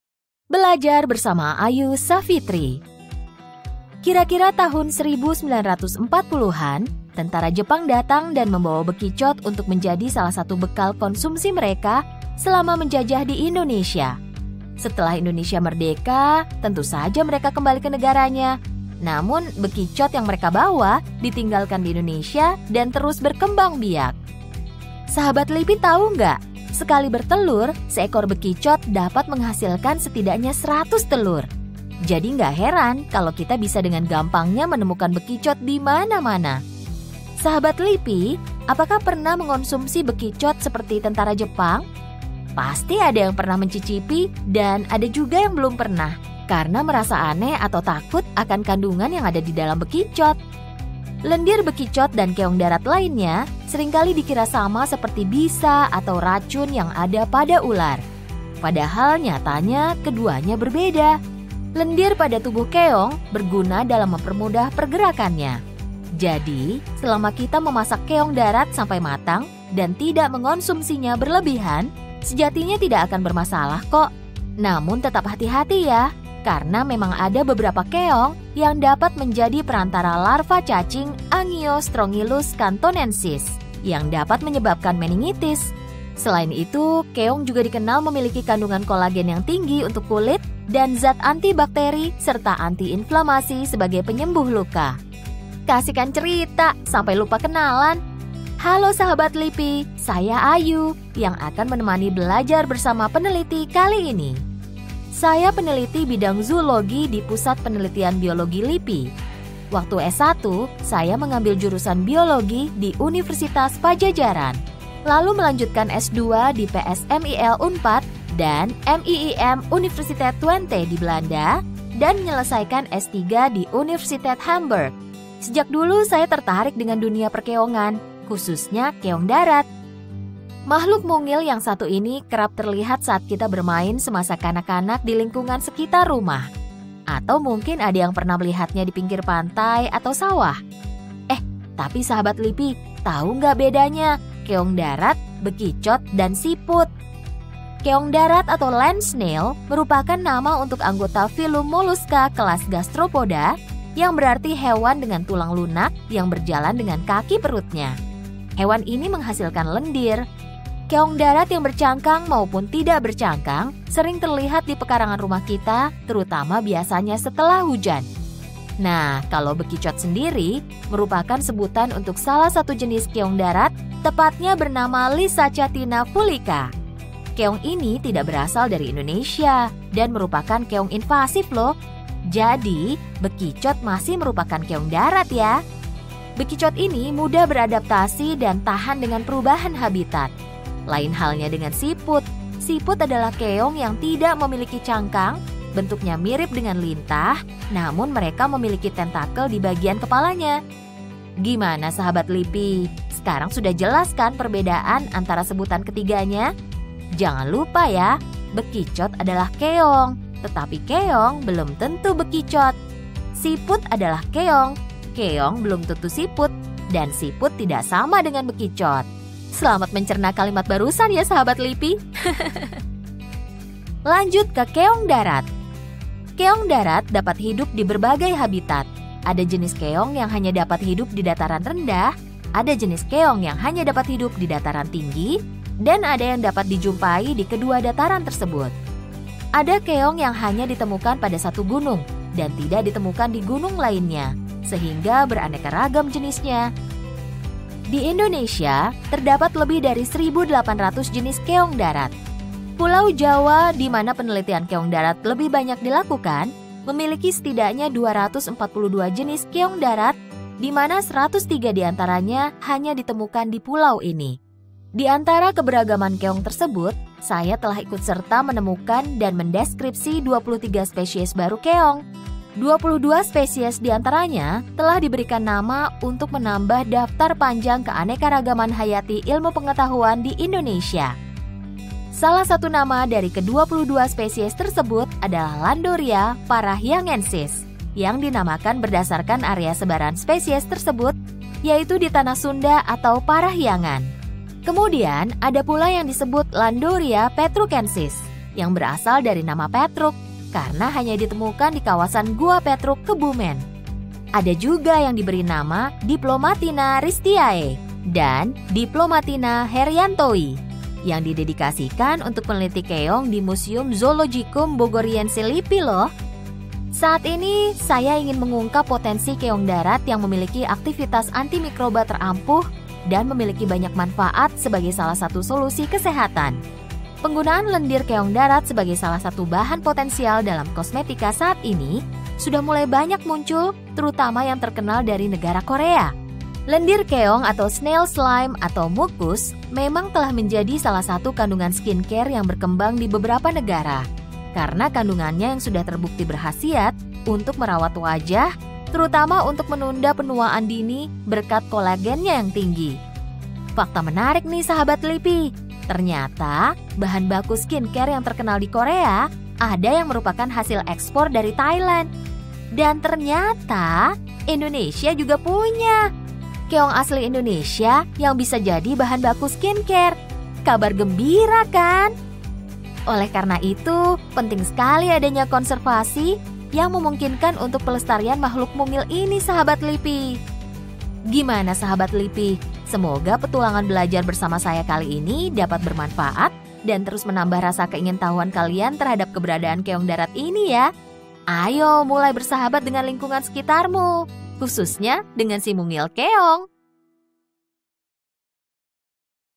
Belajar Bersama Periset Ayu Savitri Nurinsiyah: Mengenal Keong Darat Indonesia
Kawan BRIN,

Tahu gak bedanya: keong darat, bekicot dan siput?

Kak Ayu, periset dari BRIN akan bercerita tentang keong darat Indonesia. Di Indonesia, terdapat lebih dari 1800 jenis keong darat dan ternyata keong darat ini memiliki aktivitas antimikroba terampuh dan memiliki banyak manfaat sebagai salah satu solusi kesehatan. Gimana ya cerita lengkapnya, yuk tonton!

Kreator:
Ivander Jair Justus, Dara Khairunissa Krisega, Amanda Marcelia, Penny Sylvania Putri
Produksi 2021

Tentang BRIN